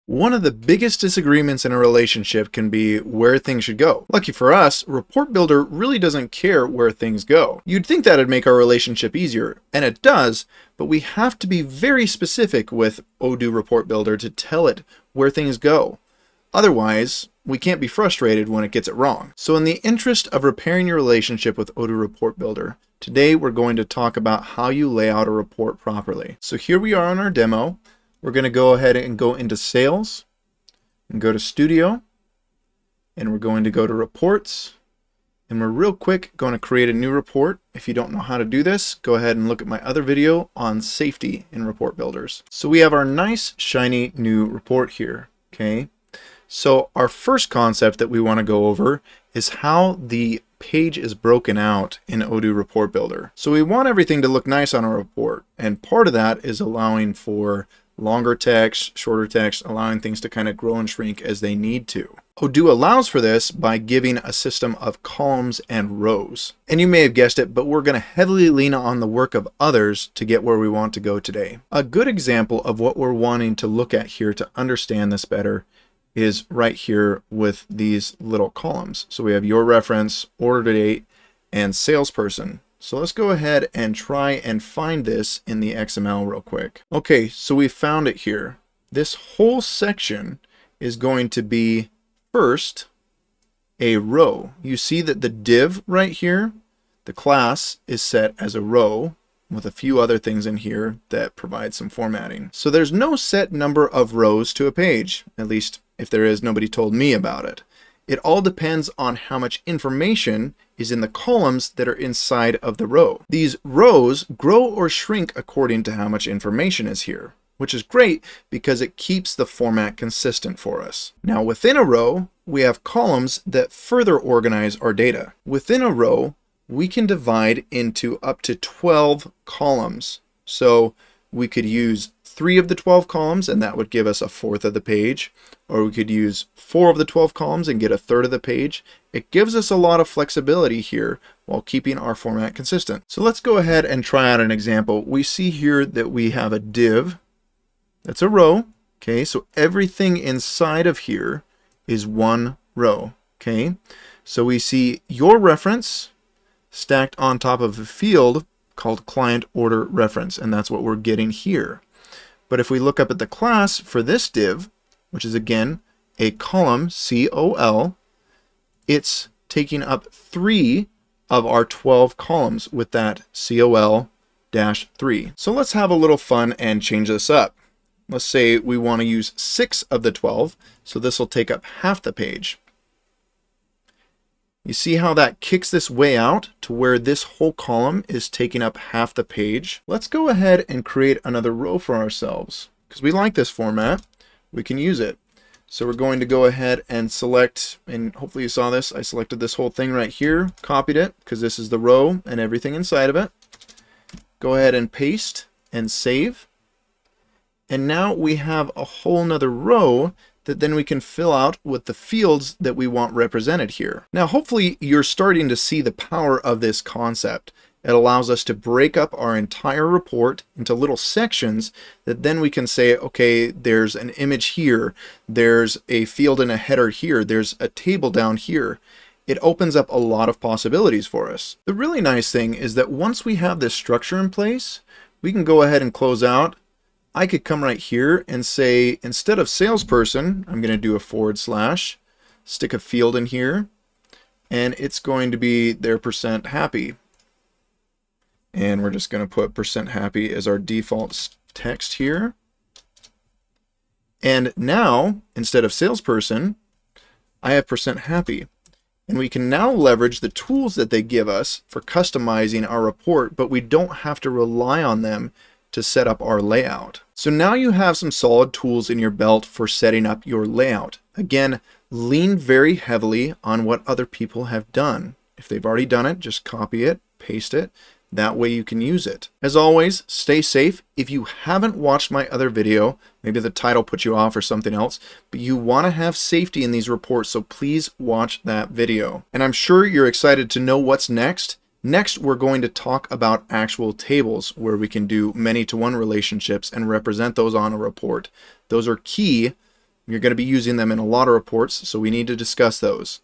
Designing the Layout of a PDF Report in ODOO V17
Now we're ready to start adjusting the layout of our report.  I would say this is probably the most important piece as the layout gives us a stable format for the entire report.  No matter what, be sure you watch this! If you want your reports to look professional, this is a key piece!